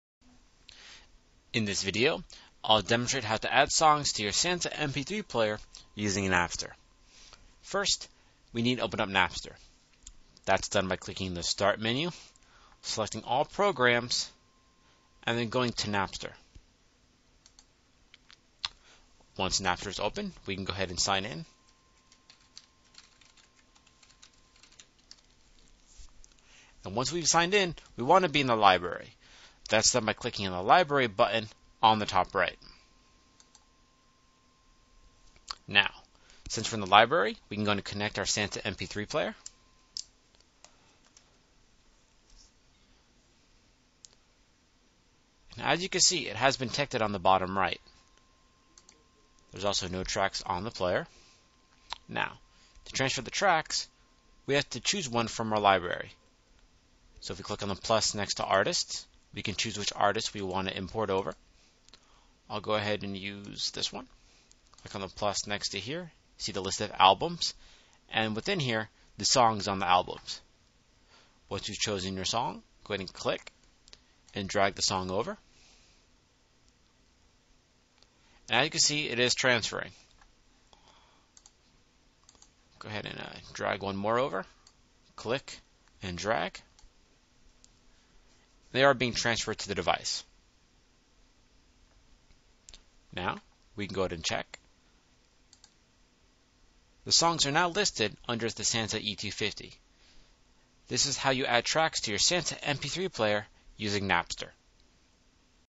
Adding Songs to Sansa MP3 Players Using Napster
How to add songs to your Sansa MP3 player using Napster.